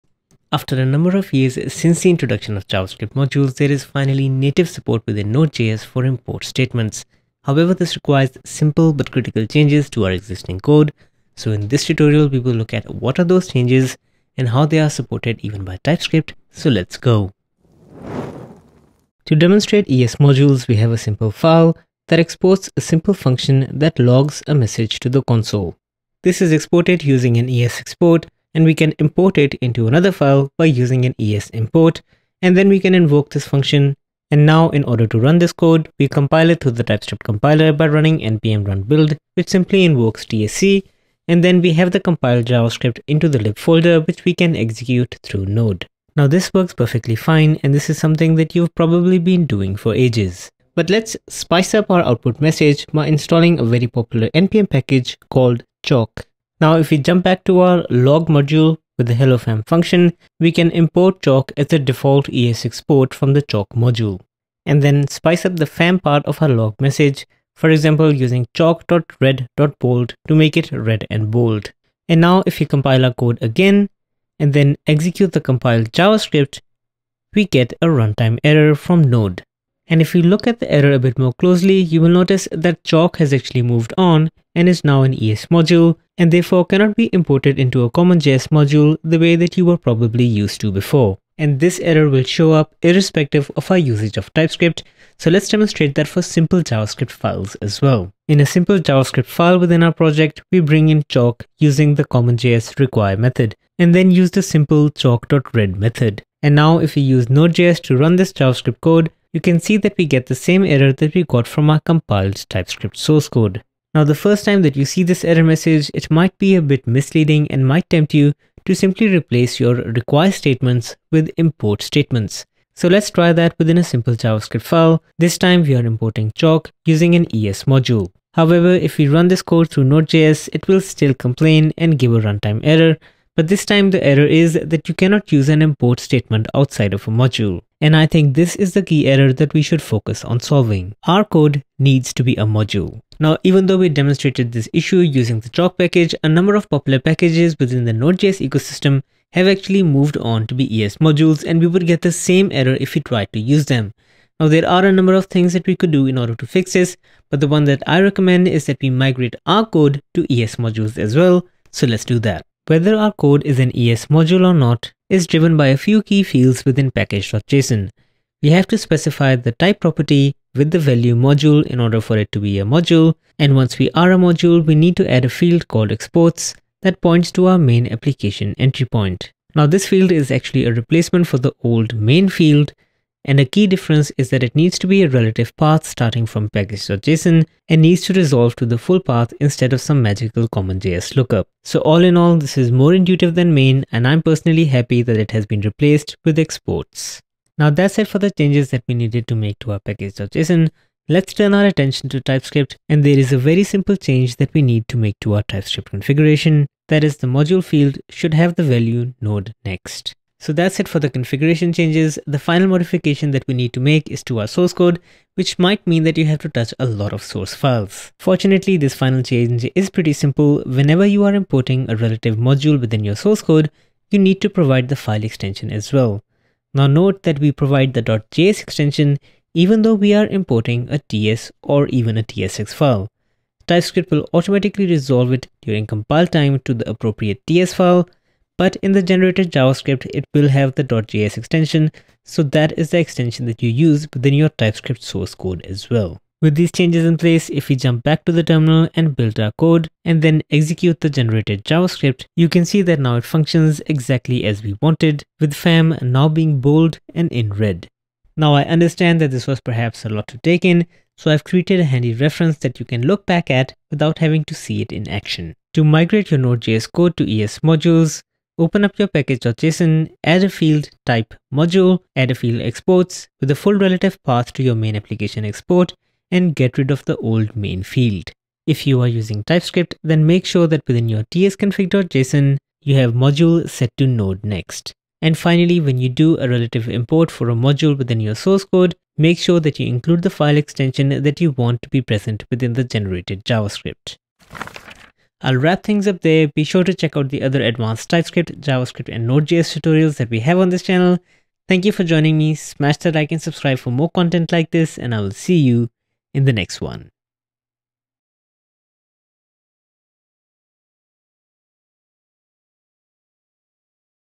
EcmaScript Modules in NodeJS // Upgrade your Projects to ES Modules
After a number of years since the standardization of JavaScript  modules nodejs finally has native support for import statements. However the way it works is by making simple but critical changes to your existing code.

So in this tutorial, we will look at what these changes are and how TypeScript also supports them 🌹

I'm Basarat, and I love helping developers. More about me:

200K contributions on Stackoverflow. Top Contributor for TypeScript.